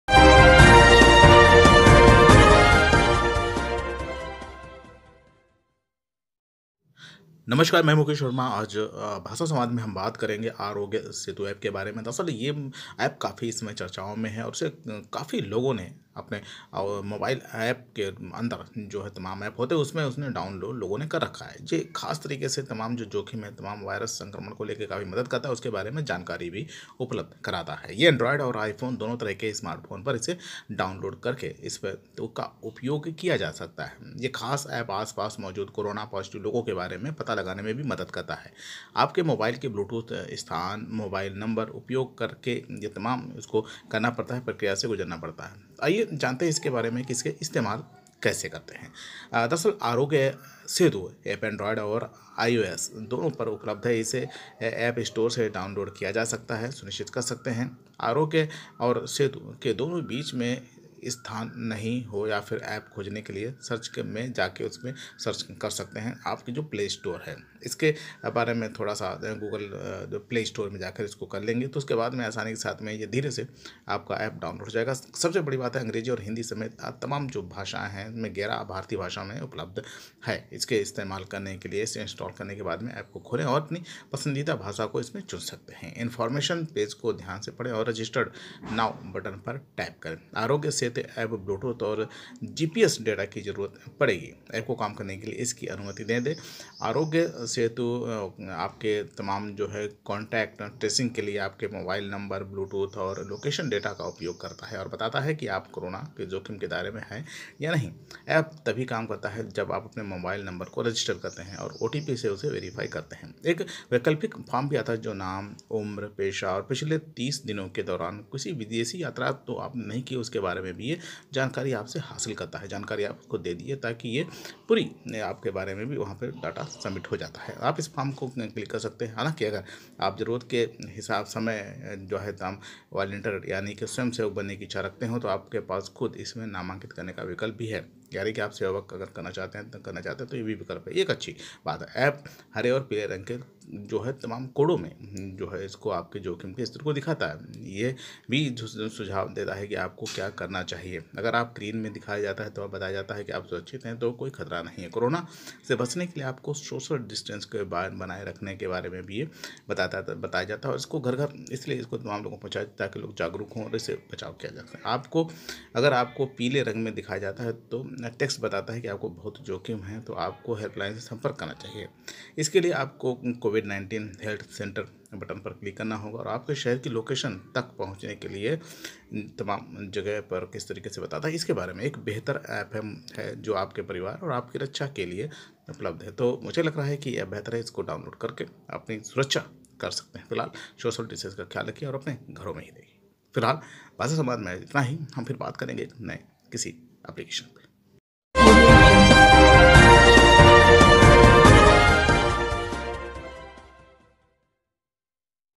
Arogya Setu app करेगा corona से लड़ने में मदद,कोरोना ट्रैकिंग आरोग्‍य सेतु का कैसे इस्‍तेमाल करें?
आरोग्य सेतु एप के लॉन्च होने के कुछ ही समय में करोङों लोगों ने इसे डाउनलोड किया है. सरकार का यह एप लोगों को कोरोना वायरस संक्रमण के खतरे और जोखिम का आकलन करने में मदद करता है.

एंड्रॉयड और आईफोन दोनों तरह के स्‍मार्टफोन पर इसे डाउनलोड किया जा सकता है. यह खास एप आसपास मौजूद कोरोना पॉजिटिव लोगों के बारे में पता लगाने में मदद करेगा. आपके मोबाइल के ब्लूटूथ, स्थान और मोबाइल नंबर का उपयोग करके ऐसा किया जाता है. आइए, देखते हैं कि आरोग्‍य सेतु एप का इस्‍तेमाल कैसे करना है...
आरोग्य सेतु एप एंड्रॉयड और आईओएस दोनों पर उपलब्ध है. इसे एप स्टोर के जरिये डाउनलोड किया जा सकता है. सुनिश्चित करें कि आरोग्‍य (Aarogya) और सेतु (Setu) के बीच कोई स्थान नहीं हो या फिर एप खोजने के लिए सर्च बार में 'AarogyaSetu' टाइप करें. हमने नीचे लिंक भी दिए हैं:
अंग्रेजी और हिंदी समेत आरोग्‍य सेतु एप 11 भारतीय भाषाओं में उपलब्‍ध है. इंस्‍टॉल करने के बाद एप को खोलें और अपनी पसंदीदा भाषा को चुनें
इनफॉर्मेशन पेज को ध्‍यान से पढ़ें और 'रजिस्‍टर नाउ' बटन पर टैप करें
आरोग्‍य सेतु एप को ब्‍लूटूथ और जीपीएस डेटा की जरूरत पड़ेगी. एप को काम करने के लिए इसकी अनुमति दें. आरोग्य सेतु कॉन्‍टैक्‍ट ट्रेसिंग के लिए आपके मोबाइल नंबर, ब्लूटूथ और लोकेशन डेटा का उपयोग करता है और बताता है कि आप कोरोना के जोखिम के दायरे में है या नहीं.
एप तभी काम करता है जब आप अपने मोबाइल नंबर को रजिस्‍टर करते हैं और ओटीपी से उसे वेरिफाई करते हैं. एक वैकल्पिक फॉर्म भी आता है जो नाम, उम्र, पेशा और पिछले 30 दिनों के दौरान विदेश यात्रा के बारे में पूछता है.

आप इस फॉर्म को स्किप कर सकते हैं. हालांकि, अगर आप जरूरत के समय में वॉलेंटियर यानी स्वयंसेवक बनने की इच्छा रखते हैं तो आपके पास खुद को इसमें नामांकित करने का विकल्प है.
एप हरे और पीले रंग के कोडों में आपके जोखिम के स्‍तर को दिखाता है. यह भी सुझाव देता है कि आपको क्‍या करना चाहिए. अगर आपको ग्रीन में दिखाया जाता है और बताया जाता है कि 'आप सुरक्षित हैं' तो कोई खतरा नहीं है. कोरोना से बच